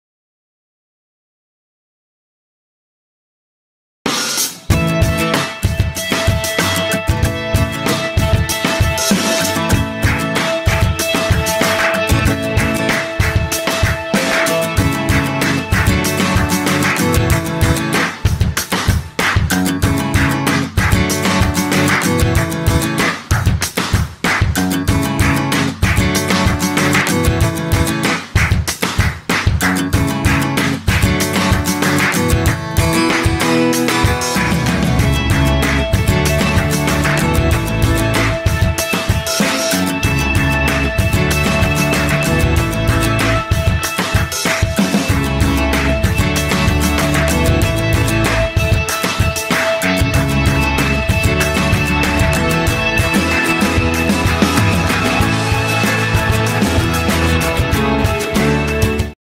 Summer Exhibition Openings - May 2016
On May 29, 2016 we celebrated the launch of our Summer exhibitions!

The afternoon featured a tour and curatorial conversation of Lucie Chan and Jérôme Havre: Liminal with curator Pamela Edmonds and Sarindar Dhaliwal: The Radcliffe Line and Other Geographies with artist Darindar Dhaliwal and curator Marcie Bronson.

Edited by Denise Apostolatos